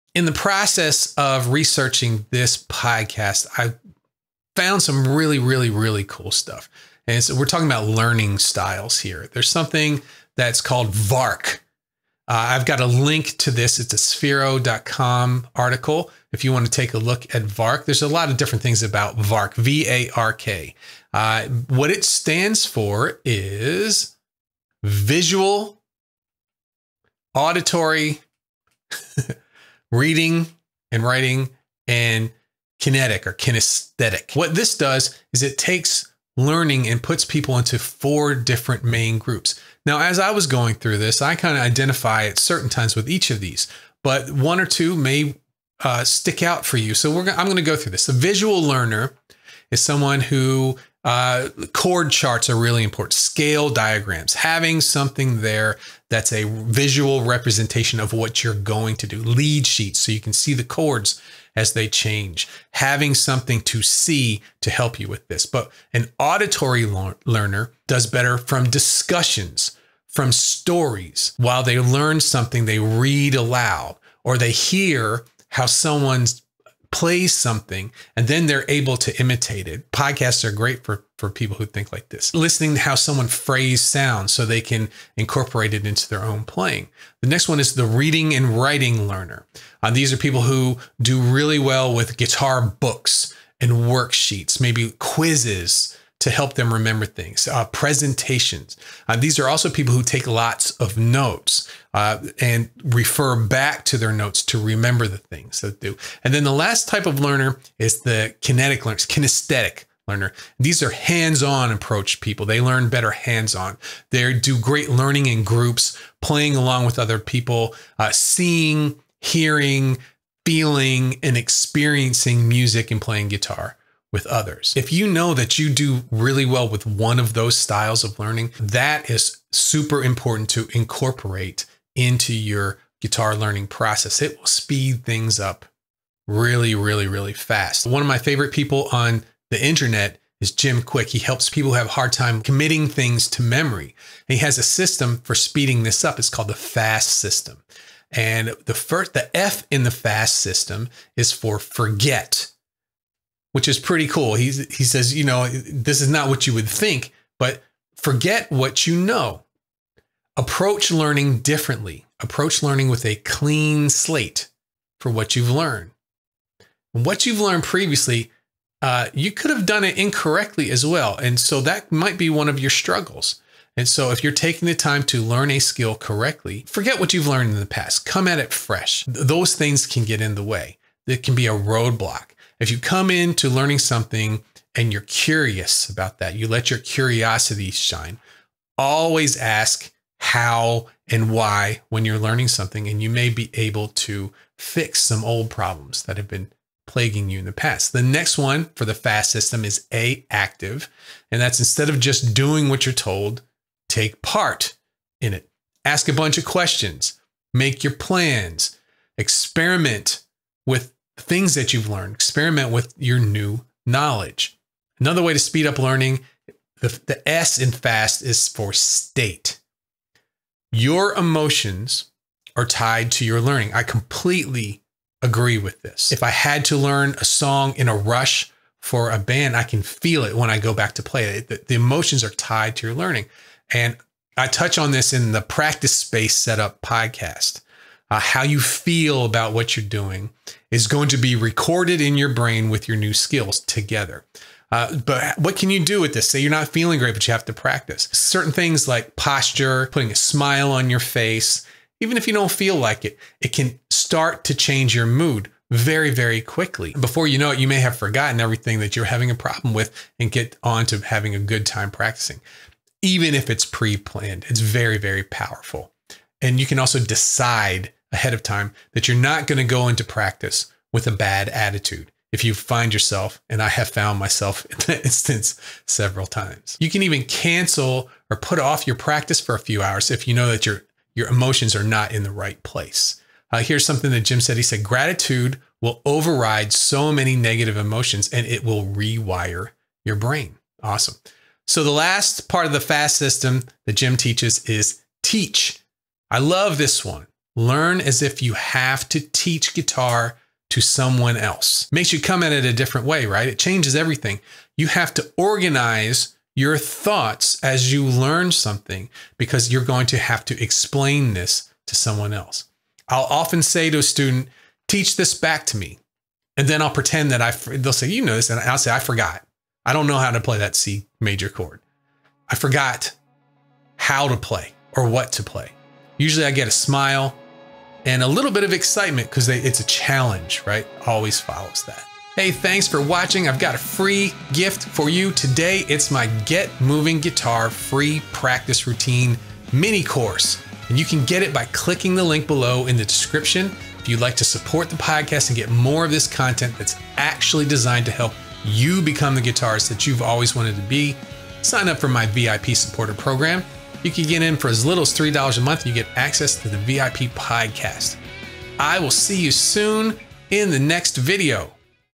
New Tips About Guitar Learning Styles
Hey, I'm Lee

Do you want to get better at guitar but you feel lost?

Today, I share the four pillars of a successful guitar learning process and some tips for speeding it up.

I end this series with some tips to help you speed up your learning time.

Check out the rest of this week's videos:

Foundational Guitar learning
The Whole Fretboard
When should I learn this?
ditch the a la carte method